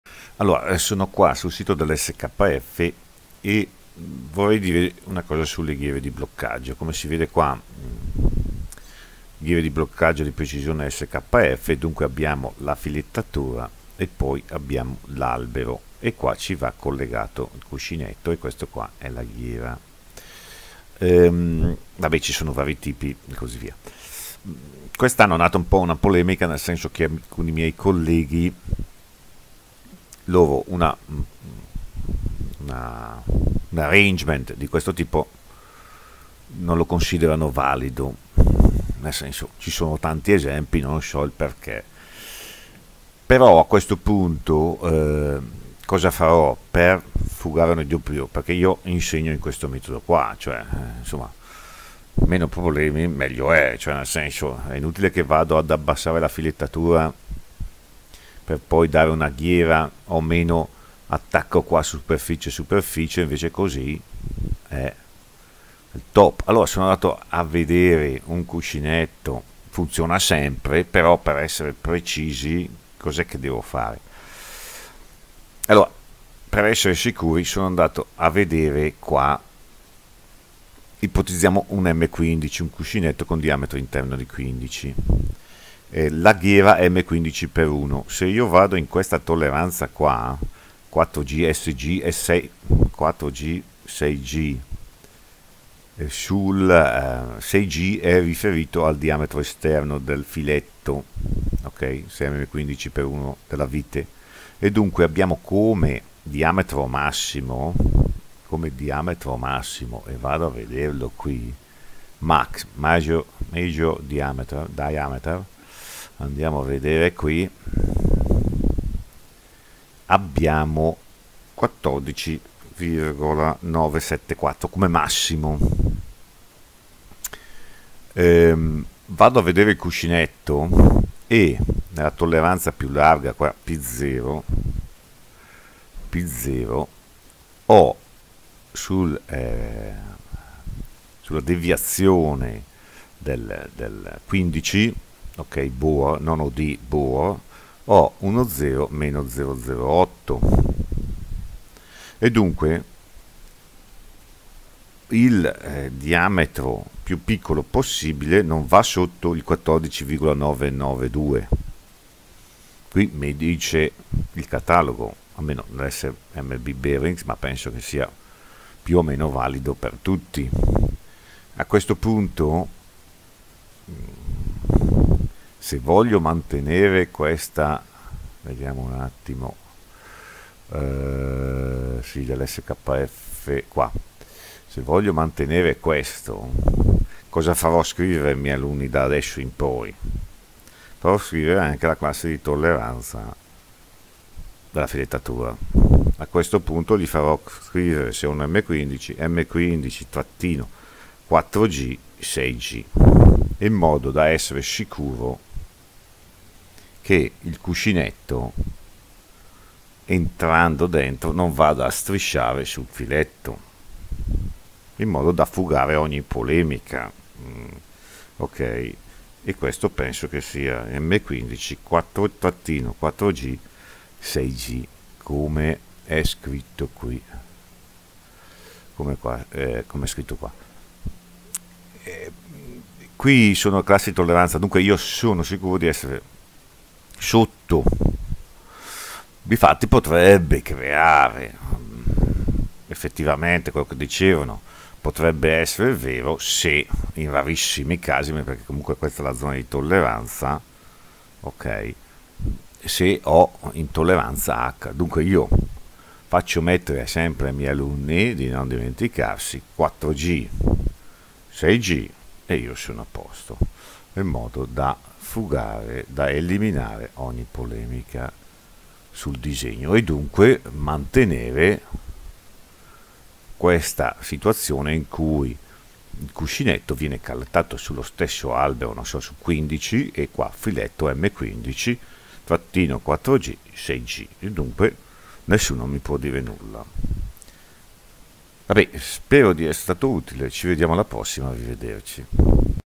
🏋️ DISEGNO GHIERA CON ALBERO BEARING NUT🏋️
I cuscinetti obliqui a sfere presentano piste degli anelli interni ed esterni spostate l’una rispetto all'altra, nella direzione dell’asse del cuscinetto. Data tale conformazione, questi cuscinetti possono sopportare carichi combinati, ovvero che agiscono contemporaneamente in direzione radiale e assiale.

La capacità dei cuscinetti obliqui a sfere di sopportare carichi assiali aumenta proporzionalmente all'aumentare dell’angolo di contatto. L’angolo di contatto è definito come l’angolo fra la linea che congiunge i punti di contatto fra sfera e piste sul piano radiale, lungo la quale il carico combinato è trasmesso da una pista all’altra, e una linea perpendicolare all’asse del cuscinetto